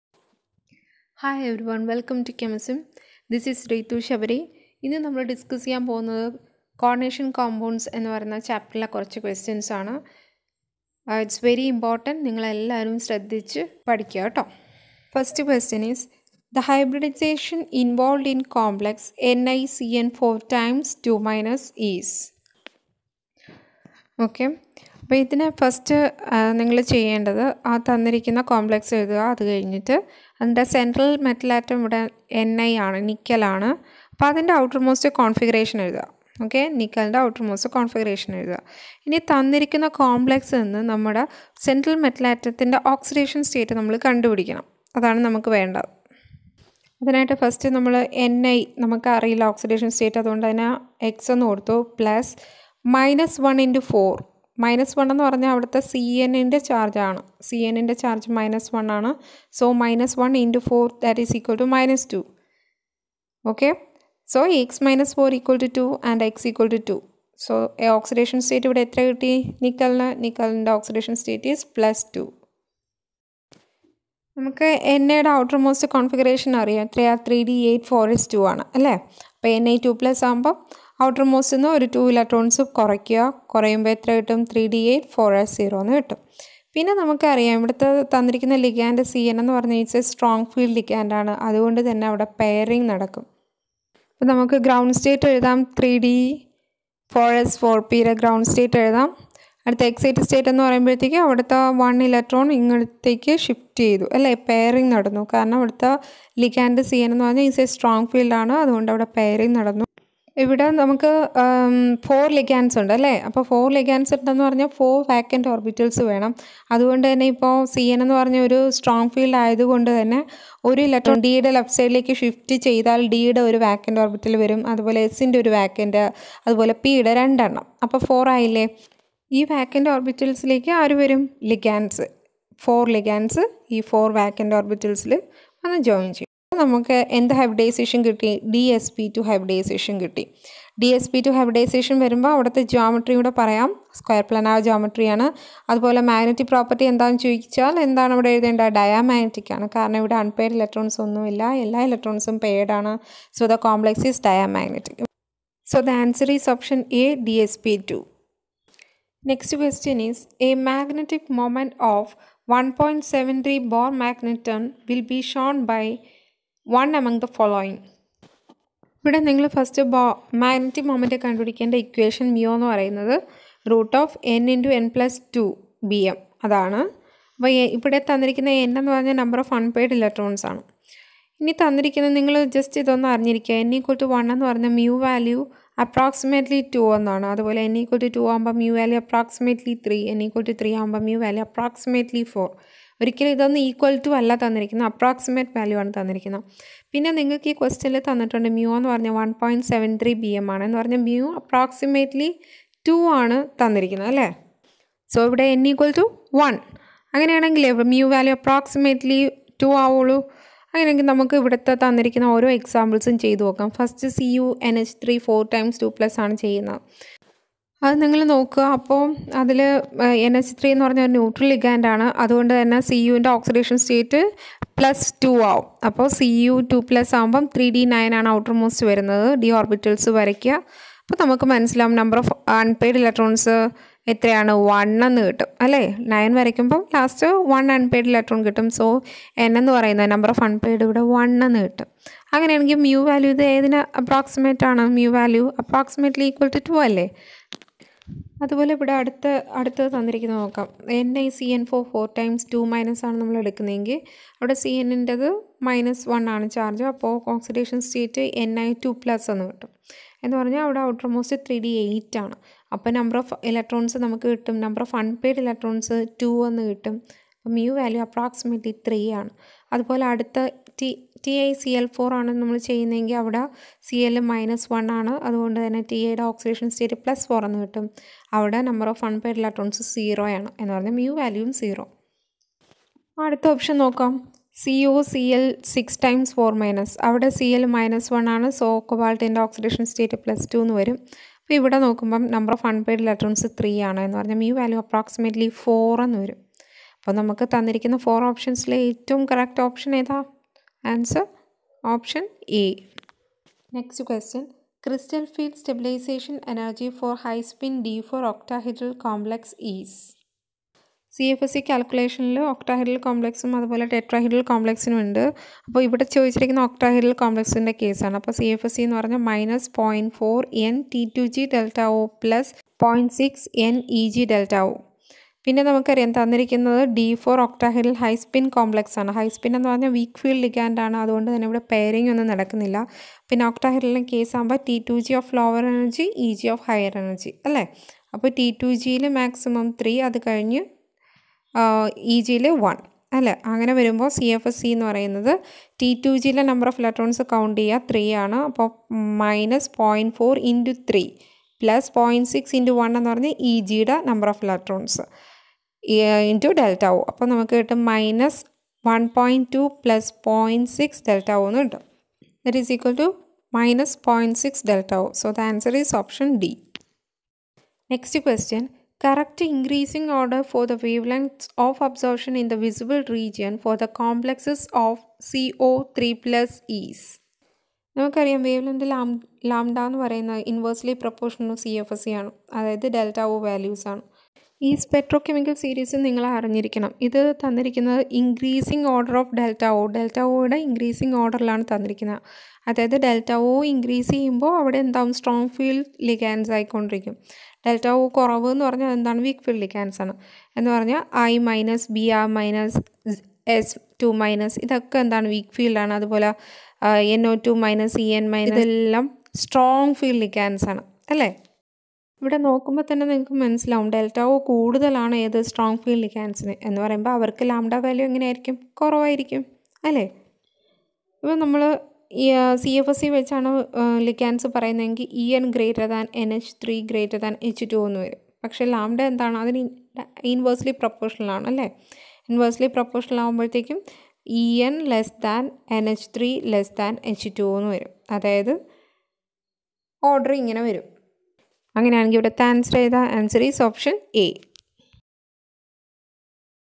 Coordination Compounds 1 | NEET/KEAM Chemistry | Trick to find Magnetic moment
Top questions from Coordination Compounds
Hybridisation of coordination compounds
Magnetic moment ( BM)
Spectrochemical series